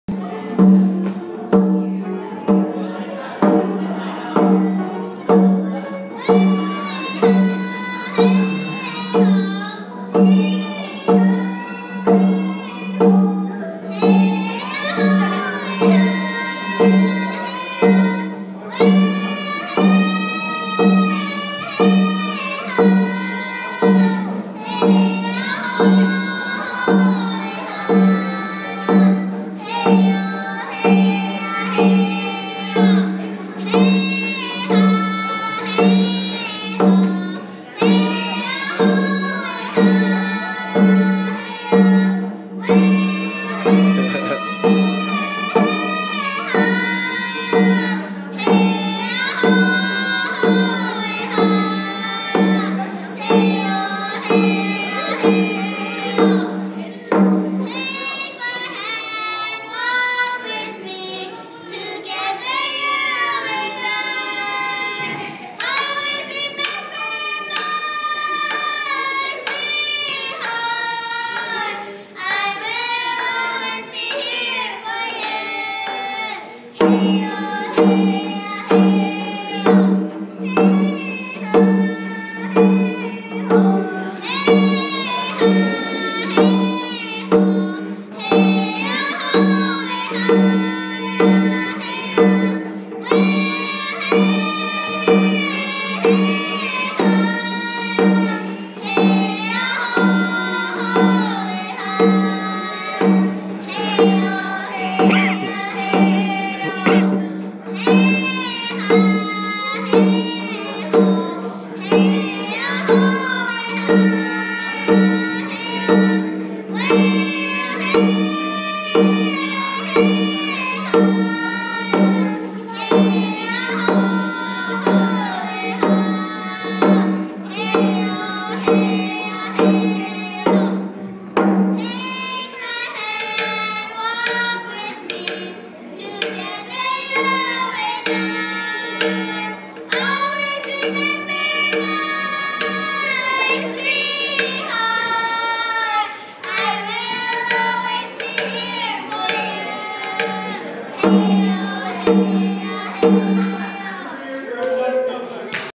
Clay hill singers 2011
Tamia & elana hope conference 2011 Lil o man drumming for them